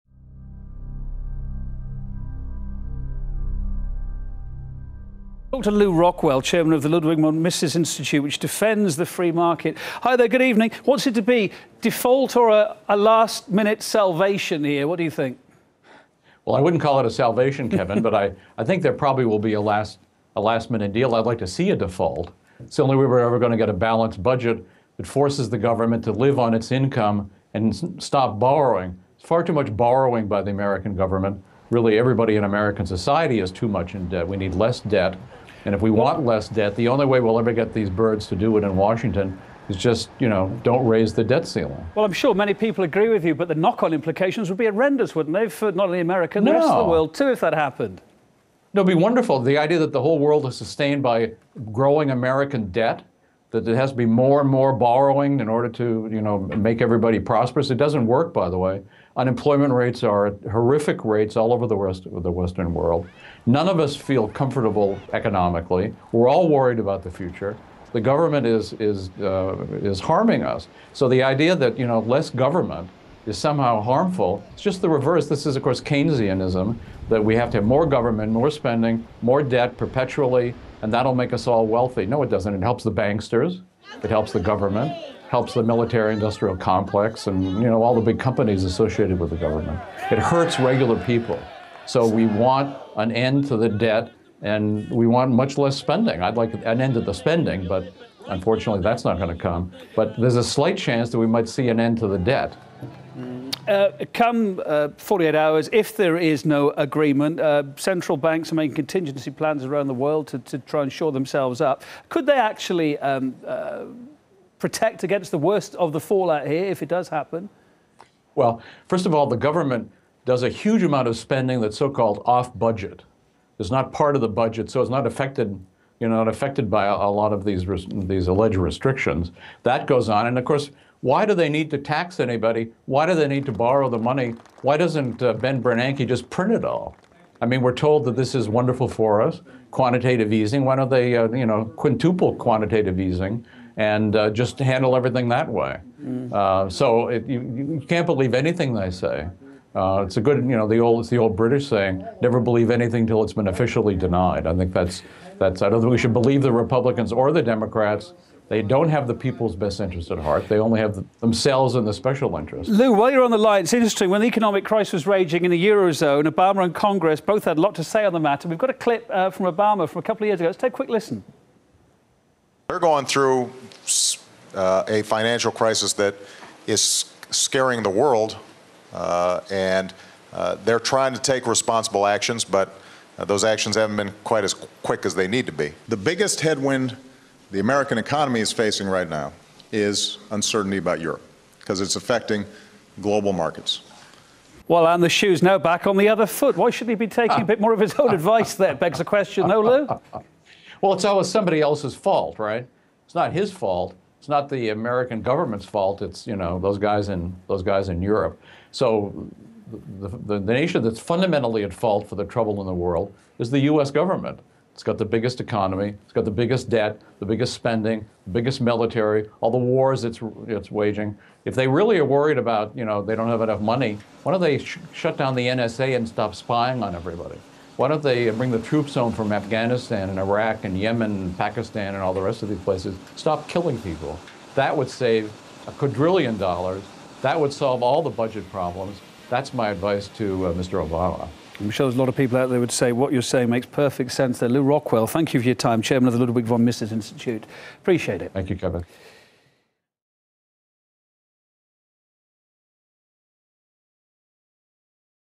The Mises View: "The Government Default" | Lew Rockwell
In his recent appearance on RT, Lew Rockwell discusses the upside of government default. Rockwell is founder and CEO of the Mises Institute.